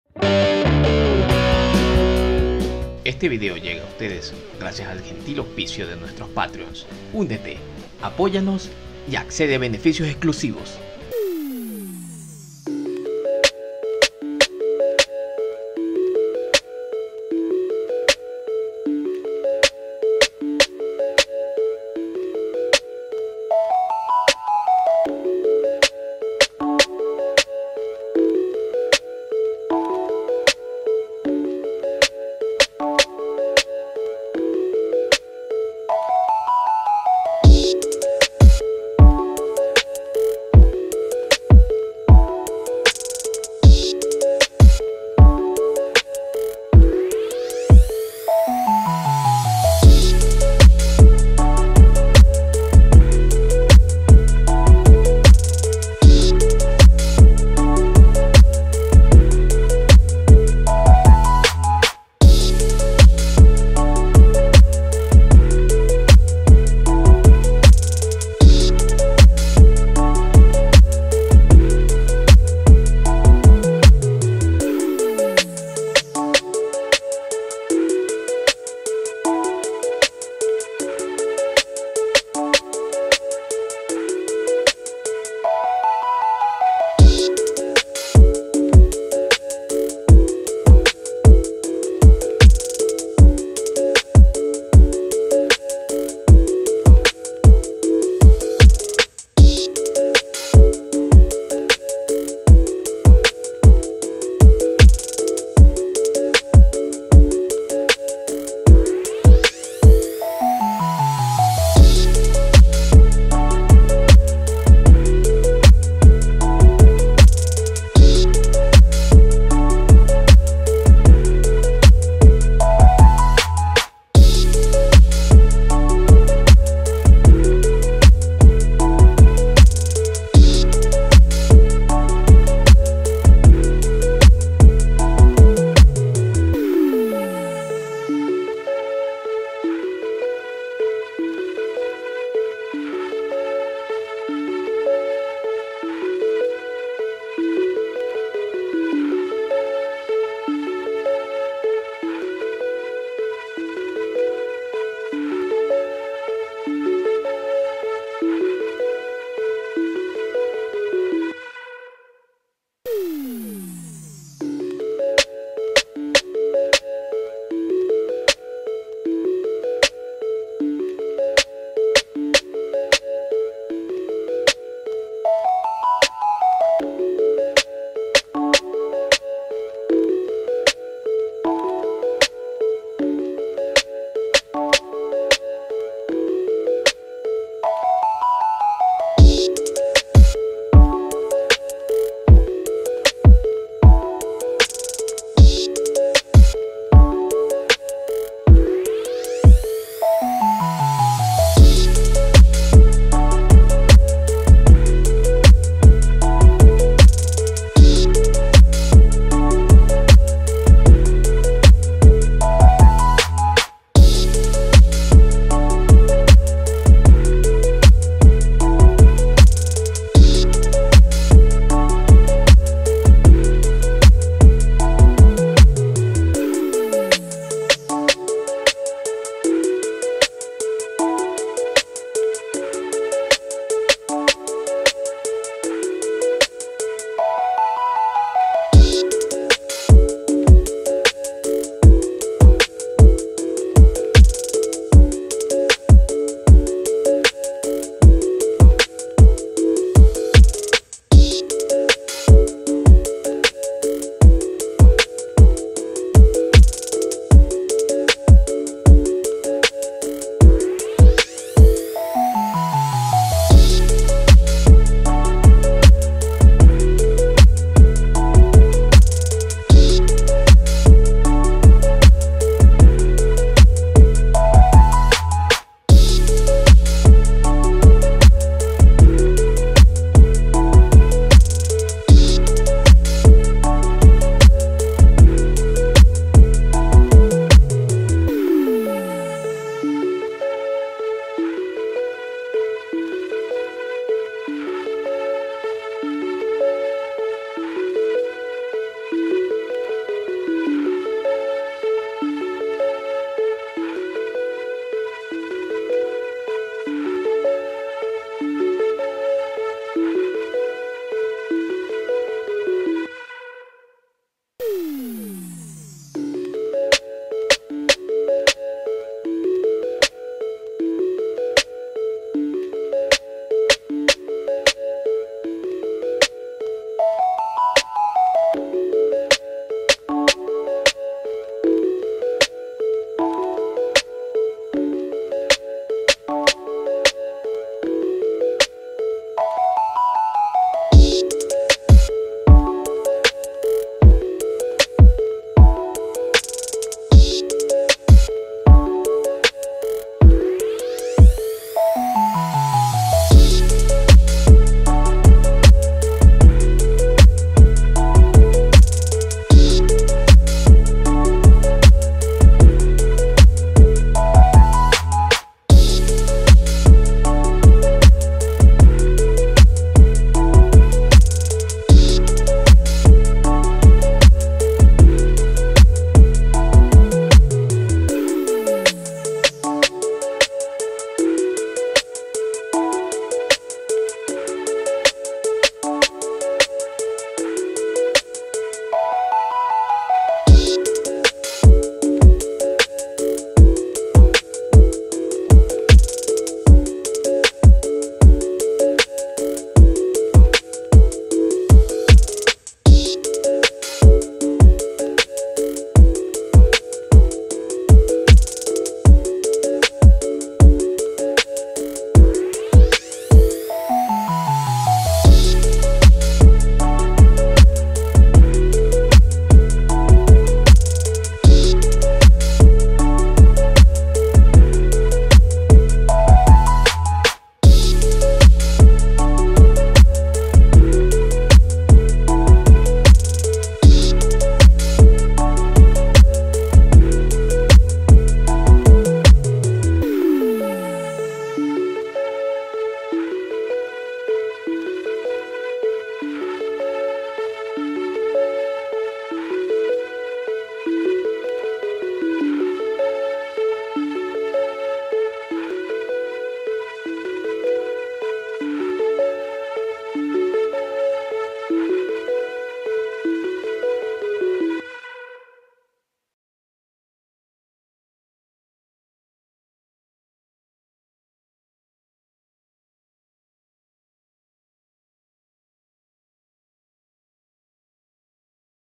EL CUERVITO FUMANCHÚ - Dibujando el comic Nro 257
CuervitoFumanchu Video del proceso de dibujo del webcómic semanal Nro 257